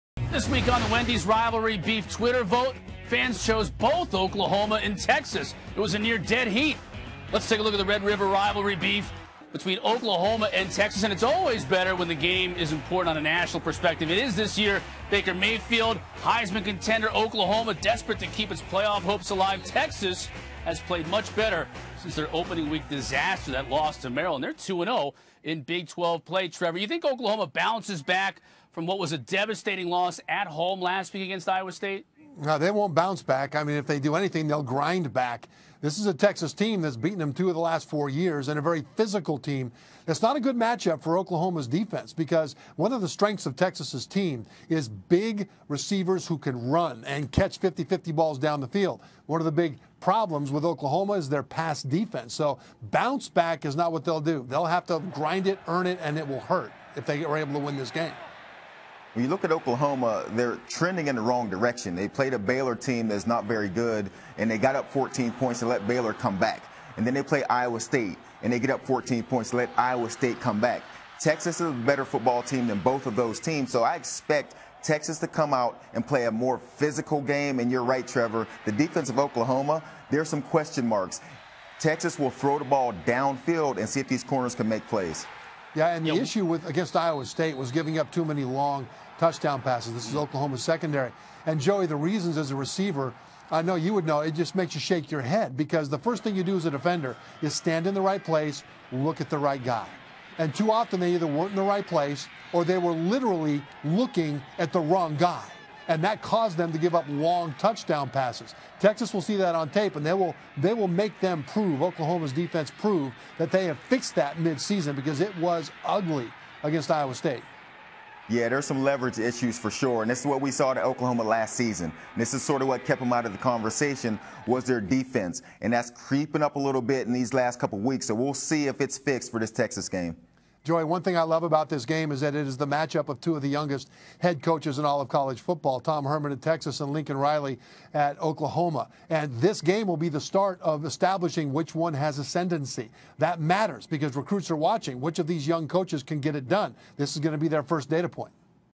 Trevor Matich says Texas is not a good matchup for Oklahoma
texas oklahomasooners longhorns football